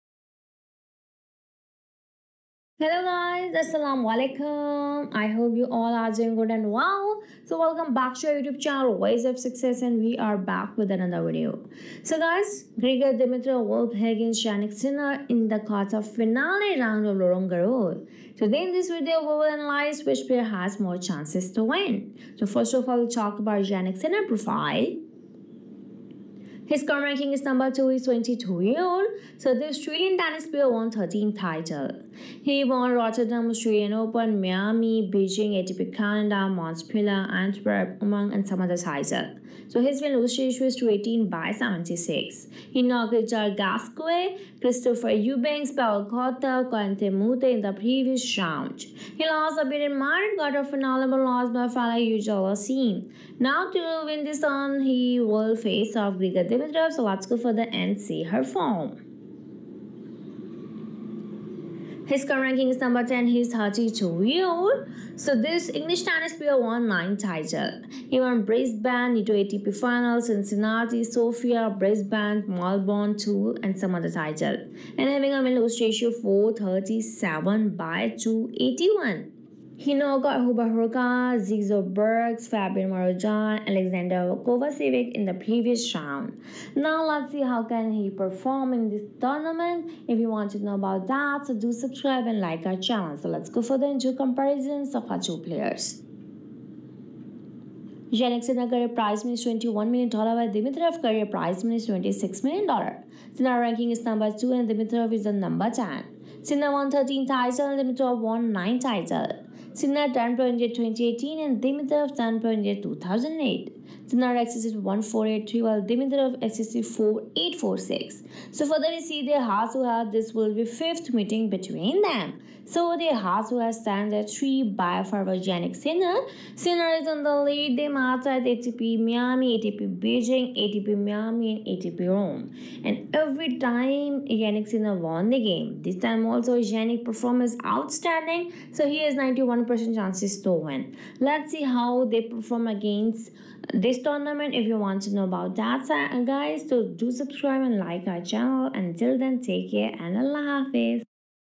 ATP LIVE JANNIK SINNER VS GRIGOR DIMITROV ATP ROLAND GARROS OPEN 2024 TENNIS PREVIEW STREAM
In this Video We Will Discuss ATP ROLAND GARROS Complete Analysis of Match Between Jannik Sinner VS Grigor Dimitrov ATP ROLAND GARROS OPEN 2024 Match.

Topics to be Discuss ATP ROLAND GARROS:
1. Players Profile
2. Players Stats
3. Head to Head Preview ATP ROLAND GARROS Meetings
4. Head to Head Comparison